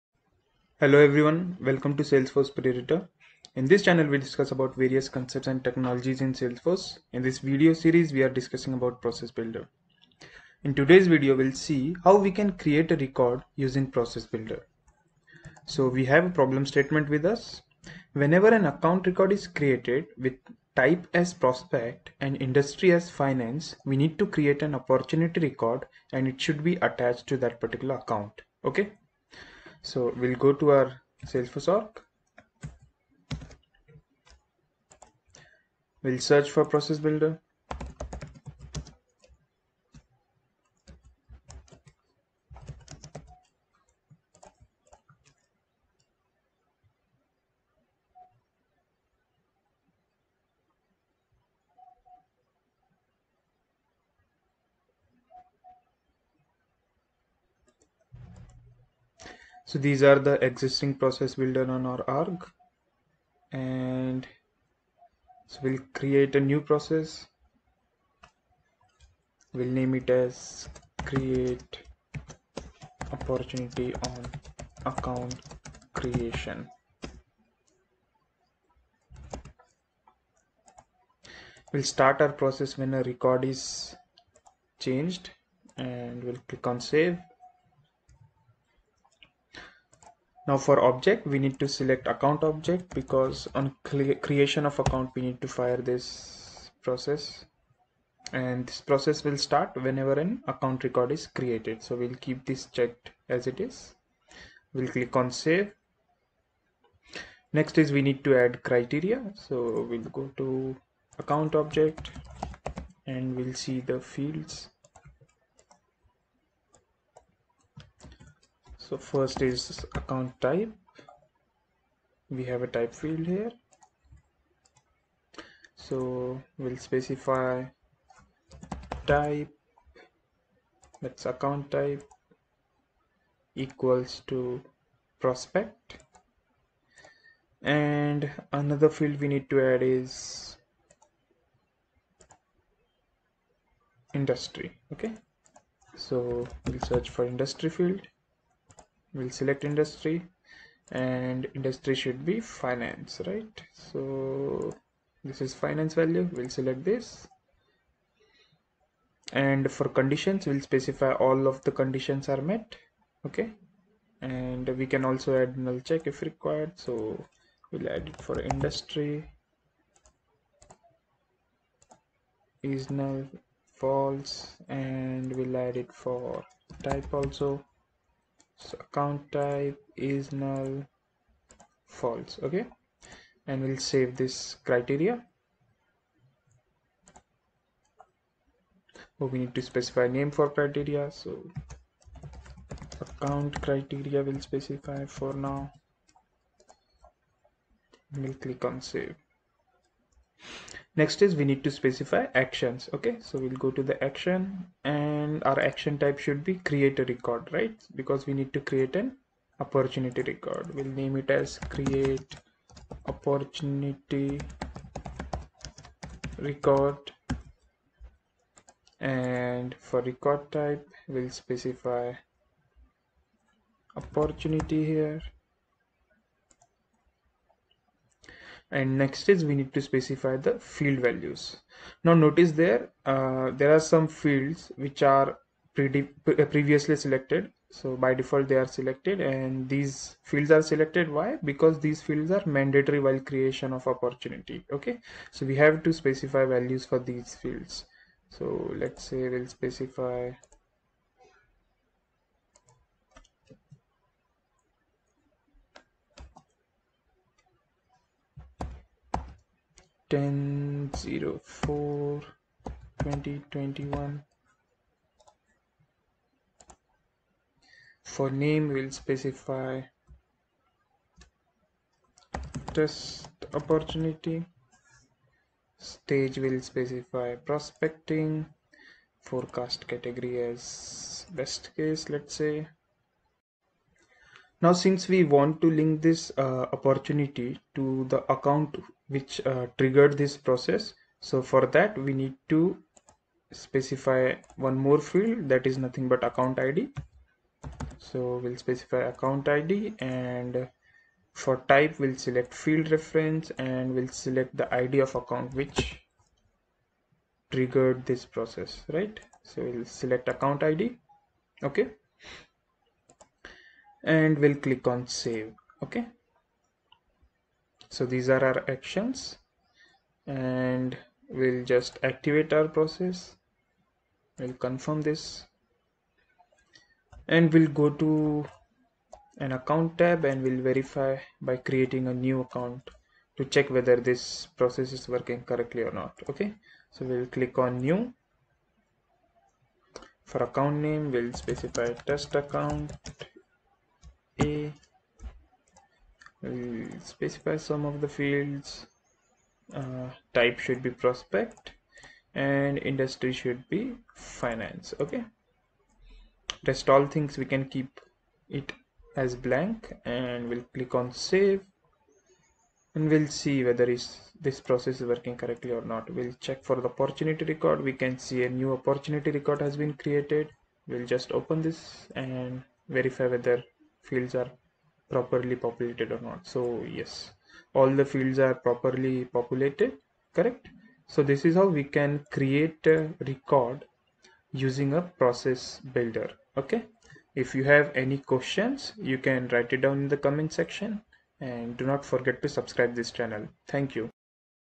Process Builder in Salesforce : How to create a Record Using Process Builder in Salesforce
In this video we discuss how we can create a record when a related record is created or update using process builder